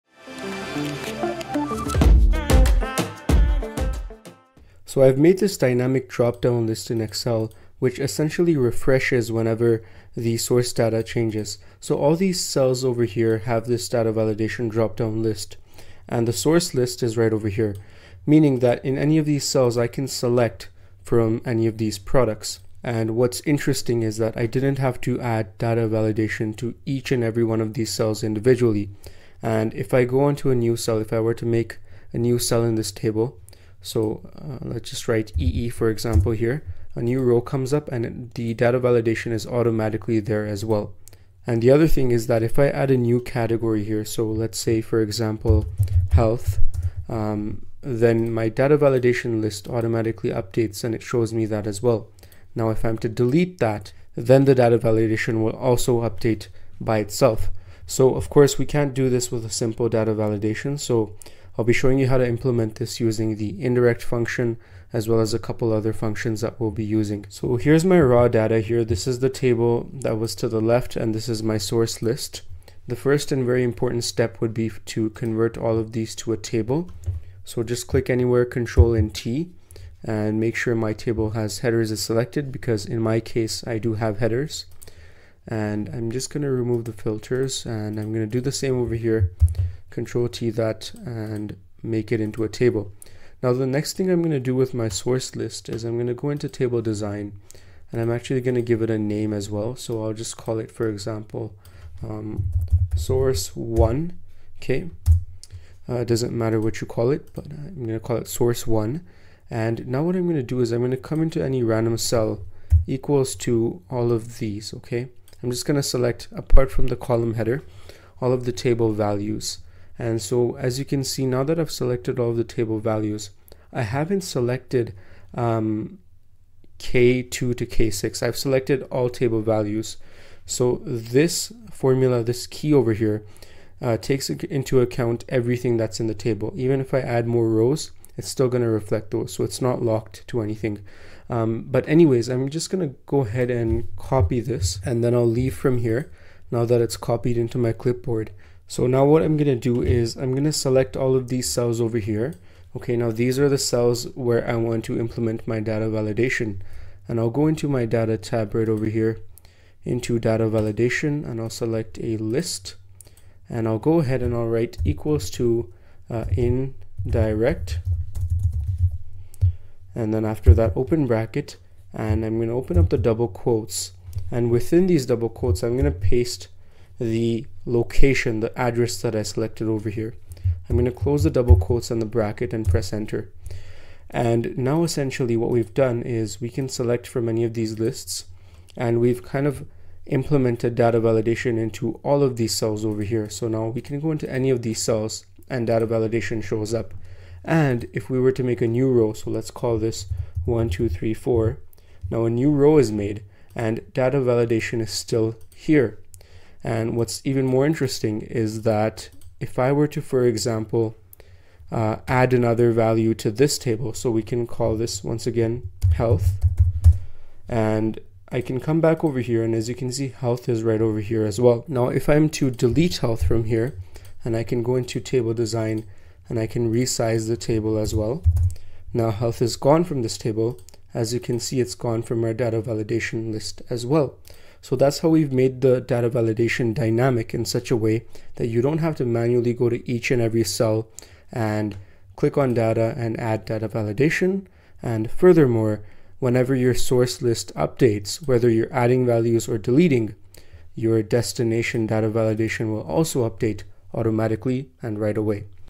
Dynamic Excel Drop-Down List in Excel that Updates Automatically!
In this Excel tutorial video, we'll explore the process of creating a dynamic drop-down list that updates itself seamlessly. Excel Drop Down lists offer a swift alternative for data entry, enhancing efficiency and reducing errors. Additionally, we'll uncover a fascinating feature allowing customization of colors for each option. Leveraging drop-down lists with functions like the INDIRECT function and data validation enables automated data retrieval, further streamlining your spreadsheet tasks.